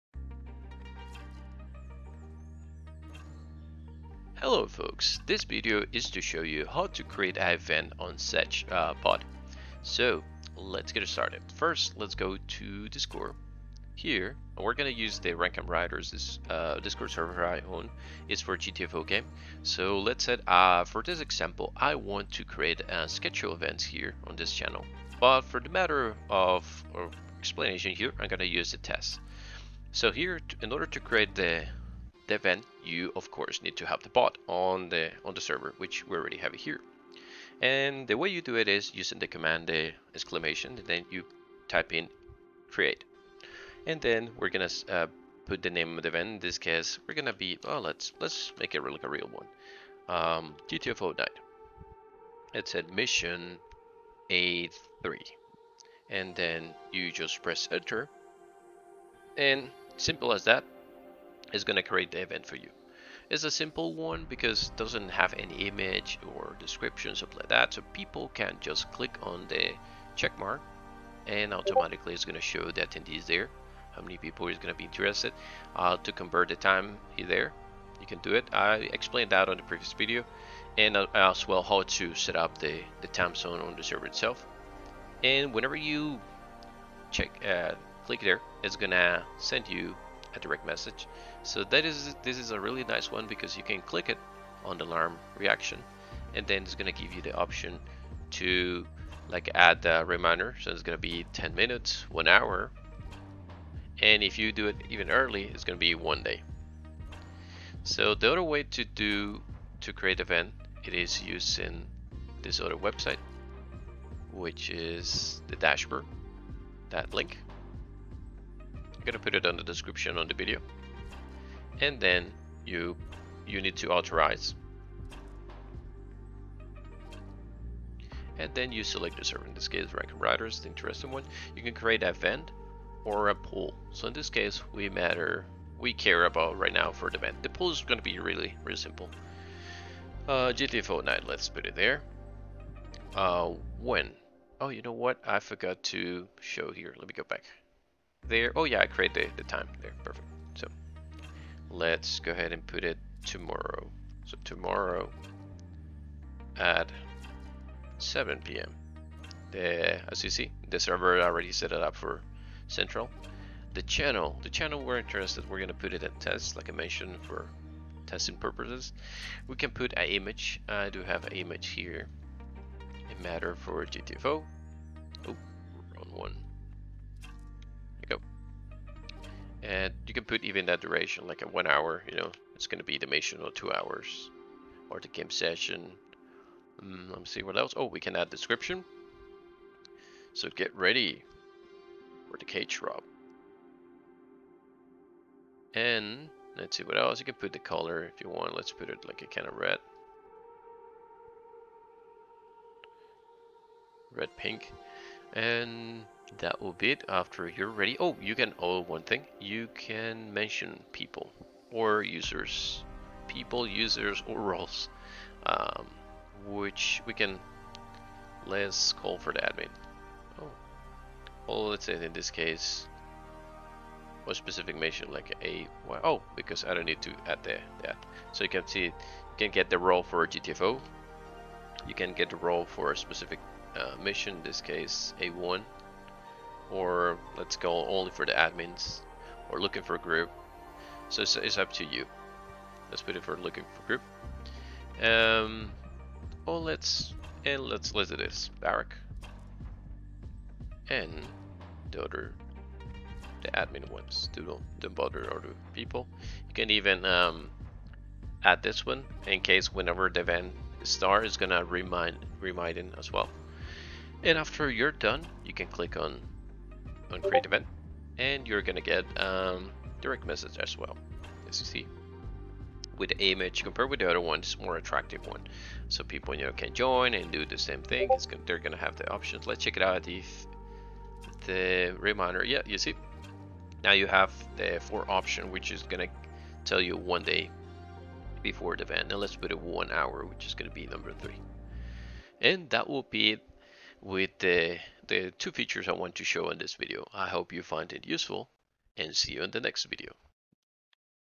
How to create an event in Sesh Bot
Here is a quick video about how to create an event using the cool sesh calendar bot I found in the past weeks to schedule game sessions for my discord servers

00:00 Intro
00:18 How to create an event (Simple, Fast method)
01:47 How to create an event (Fancy, Cool method)

Here is the link to create events using the Sesh Dashboard as I showed in the video:

If you are interested to join any of my discord servers, here are the links about it: